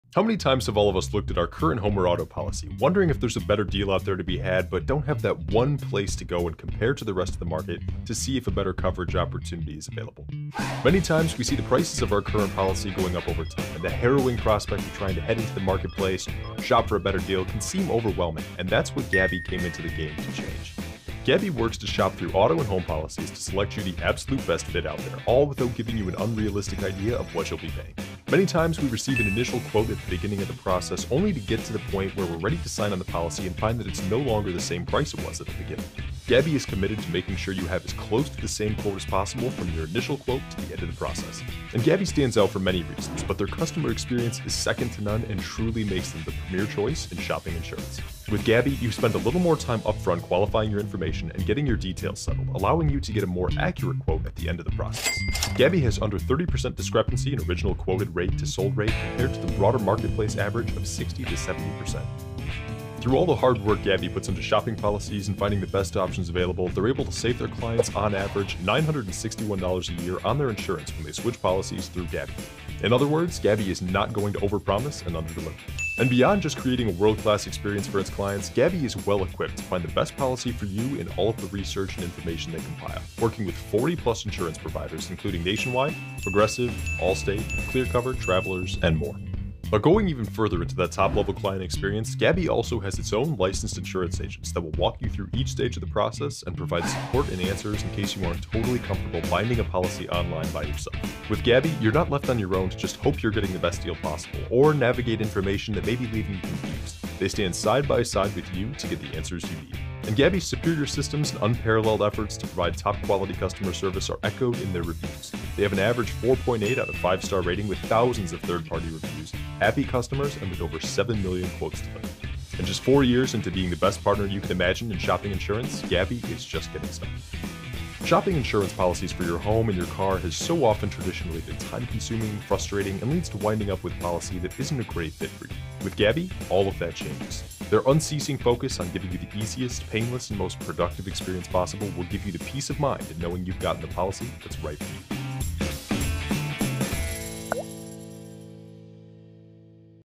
Get Better Insurance for Less! | Gabi
Technology has made it easy to find the right car, house, hotel, flight, or pretty much anything else in minutes. So why does finding the right insurance have to be so hard? We knew there had to be a better way.

Gabi’s technology is able to do in minutes what it would take you hours to do on your own. Using the account credentials to your current insurance account (or a PDF of your policy declaration), Gabi pulls up all the necessary details to match your existing coverage for an apples-to-apples comparison. We’ll get quotes from over 40 top insurance companies to help you find the best rate for your insurance needs.

Founded in 2016, Gabi has raised over $39 million in venture capital funding from top investors including Canvas Ventures, Mubadala Ventures, Northwestern Mutual Future Ventures, Correlation Ventures, CUNA Mutual Group, Securian Ventures, A. Capital Ventures, Project A, and more.
The Benzinga Brand Studio produces longform video content on behalf of partners.